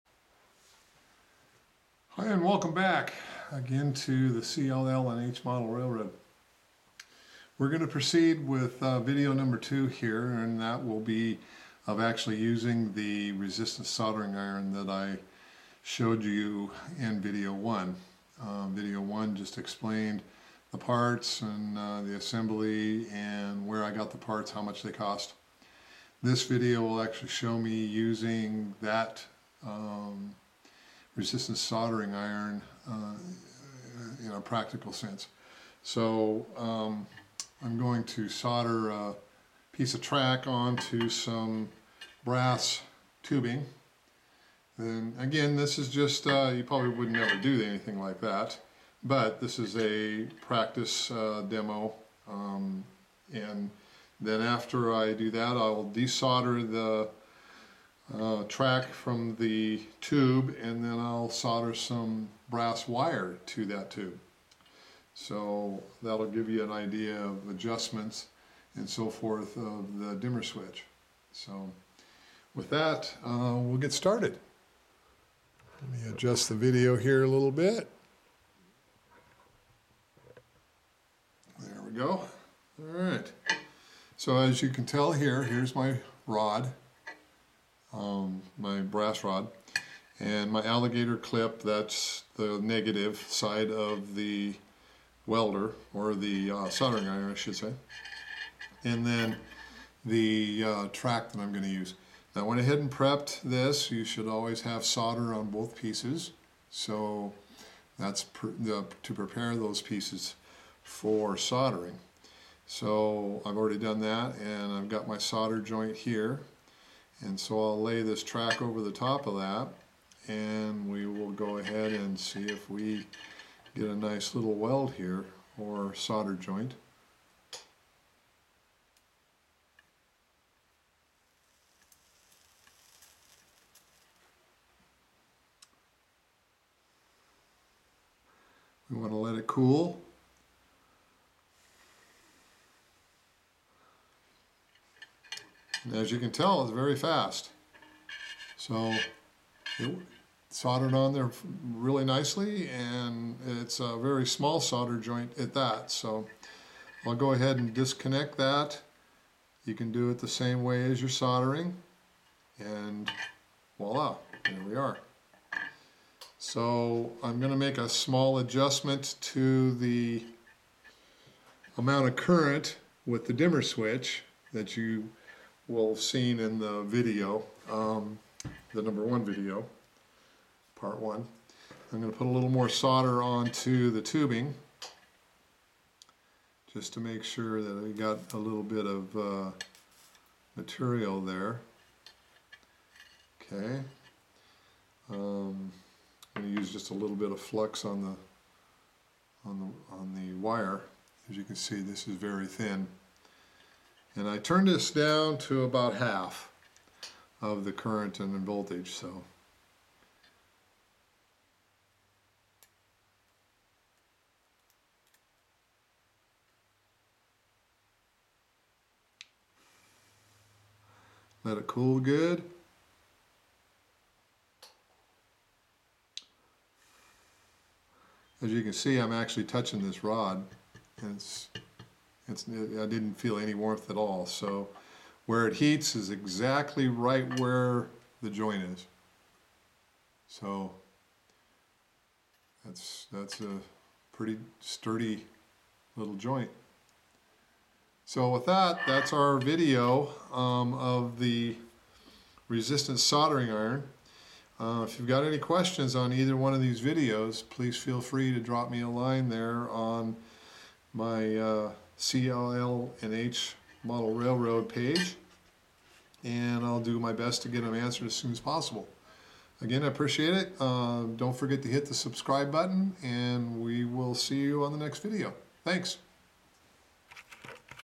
video 2 of Resistance soldering iron
In this video I'll demonstrate the Resistance Soldering Iron shown in video #1